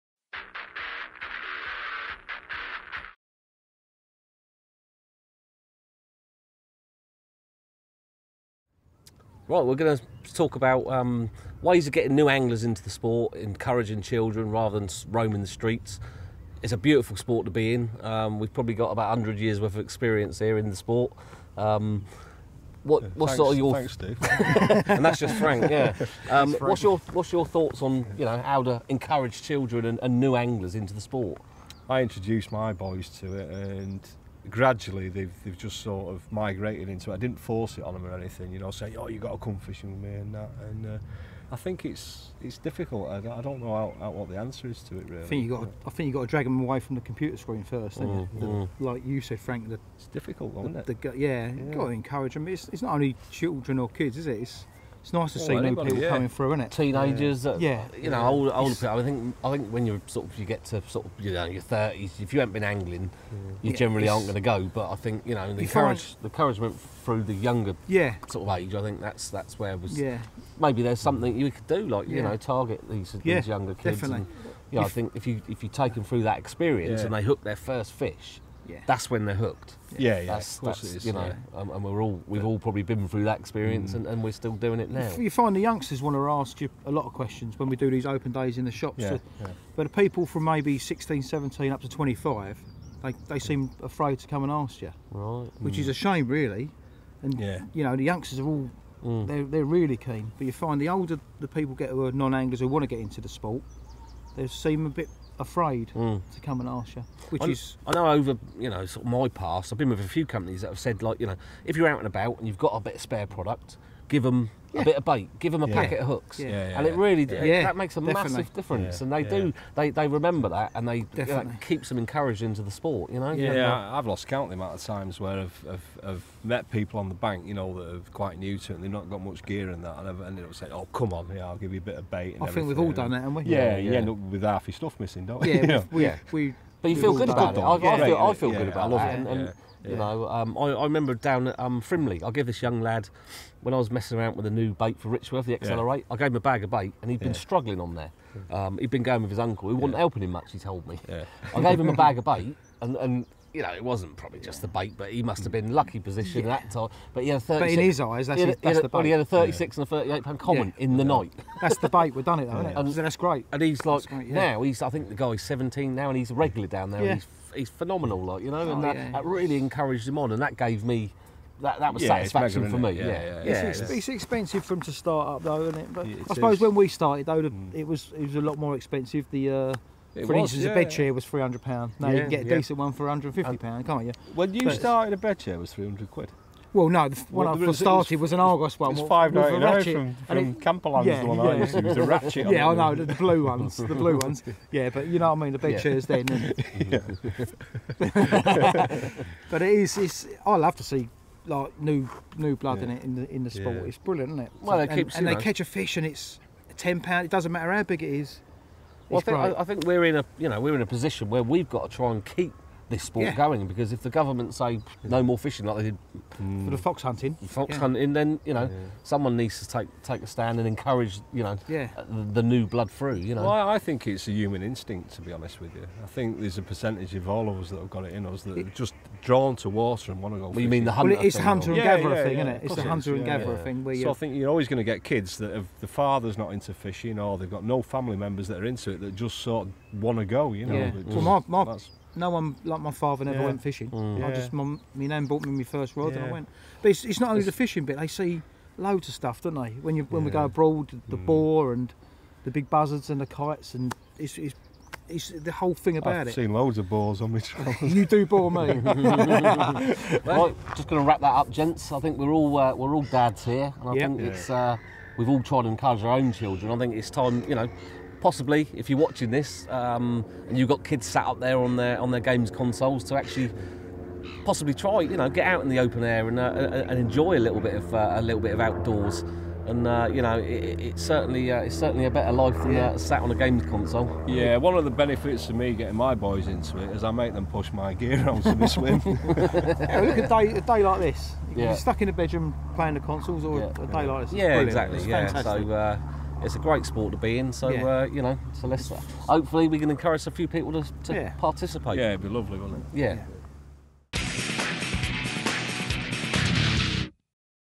Reaching New Anglers - Bivvy.TV
Steve Renyard, Frank Warwick and Lee Merritt talk about how to reach new anglers and attract them to the sport.